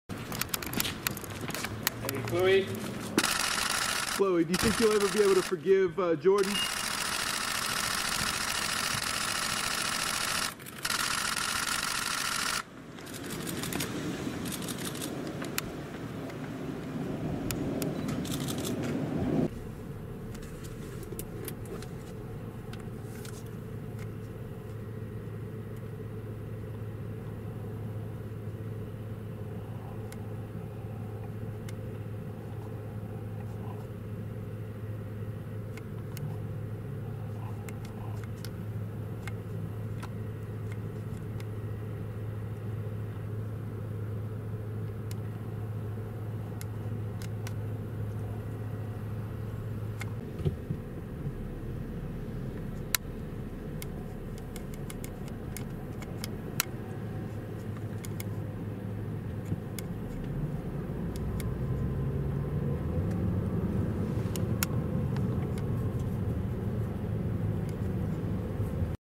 Khloe Kardashian Storms Off When Asked About Forgiving Jordyn Woods For Cheating W/ Tristan Thompson
KhloeKardashian Storms Off When Asked About Forgiving JordynWoods For Cheating With TristanThompson 2.27.19 - TheHollywoodFix.com